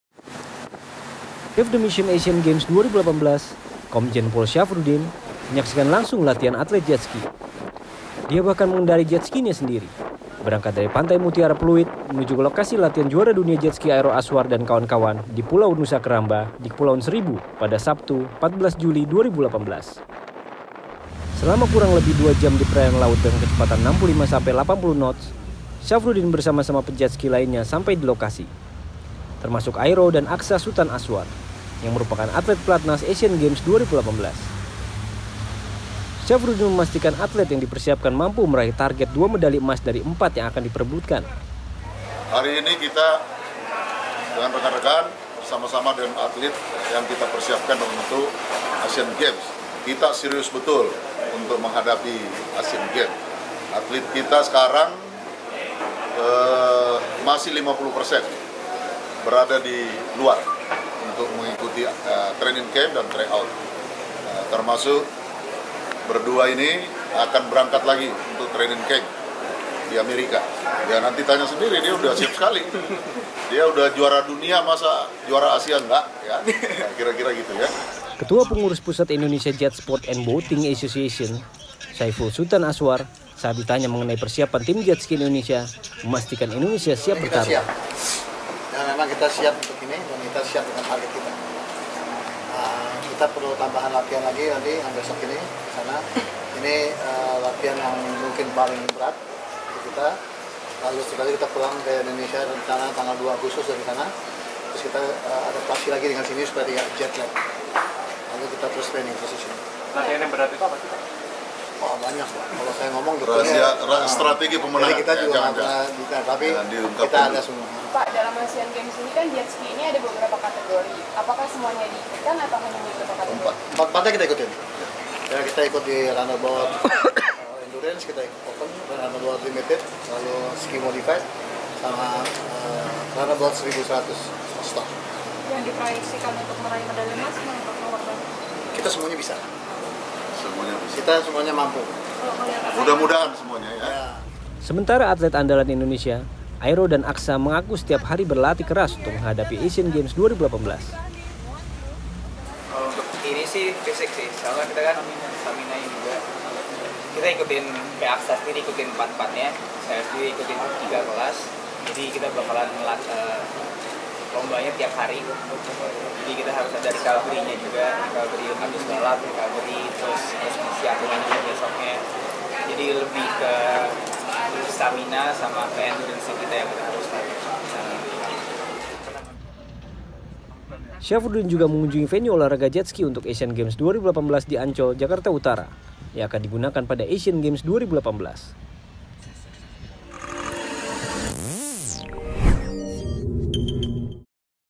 Chef de Mission Indonesia, Komjen Pol Syafruddin MSi Tinjau Atlet dengan Ber-jetski
Komjen Pol Syafruddin menyaksikan langsung latihan atlet Jetski. Dia bahkan mengendarai Jetski-nya sendiri dari Pluit ke Pulau Nusa Keramba, Kepulauan Seribu

Download aplikasi VisualTV.Live:
Akses dan Follow Official Account VISUALTV.LIVE di:
- Line: @visualtvlive
- Path: @visualtvlive